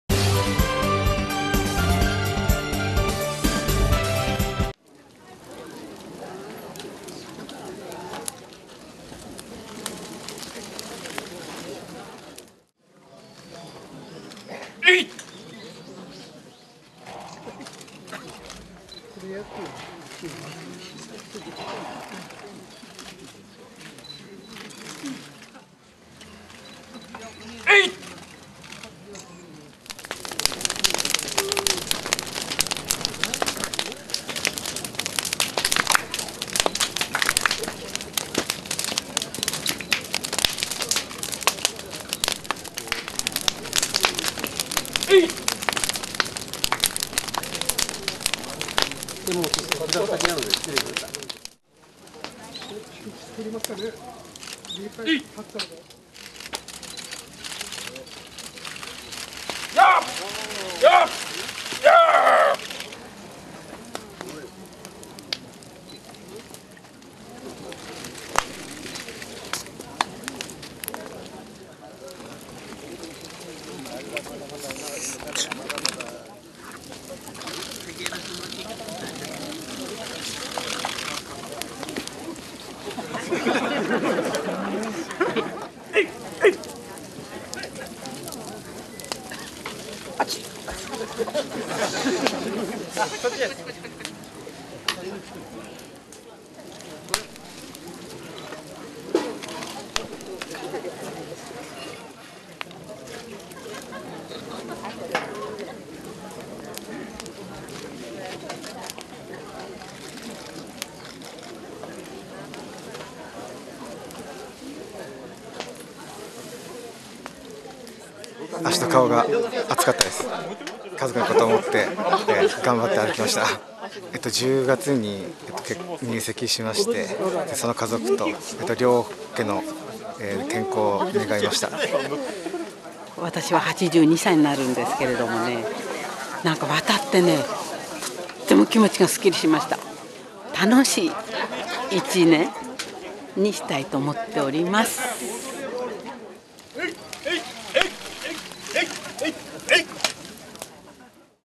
火の道歩き厄払い 火渉祭 加波山神社本宮
桜川市真壁町長岡の加波山神社本宮（宮本昌明宮司）で22日、冬至の伝統行事「火渉祭」が行われた。参加者たちが寒空の下、燃え盛る薪にできた〝火の道〟を素足で歩き、無病息災を祈願した。

火渉祭は、室町時代から続く奇祭。境内に積み上げられた薪に火が付けられると、白装束の修験者が神事を行い、薪をかき分けて道を作った。参加者は緊張した面持ちで、次々と火の中を通り抜けた。
毎年参加しているという東京都品川区、不動産業、作田耕一さん（38）は「渡る前は緊張したが、気が引き締まった。来年は良い事がありそう」と期待した。埼玉県白岡市、畑中チヤさん（73）は「来年も家族みんなが健康で過ごせればいい」と話した。